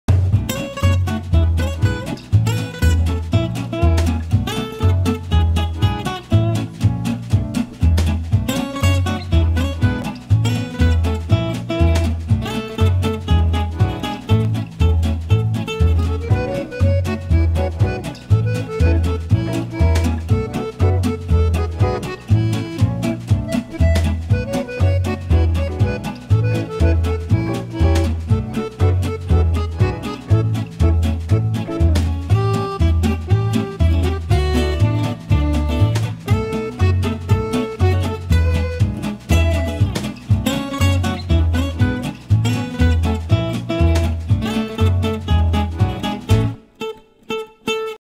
Dog Friendly Holidays - Peggy Explore Cornish Beaches!
Why staying at Sara's Cottage in Cornwall is the dream destination for dogs! See just how much fun Diane's own dog Peggy has exploring the local beaches!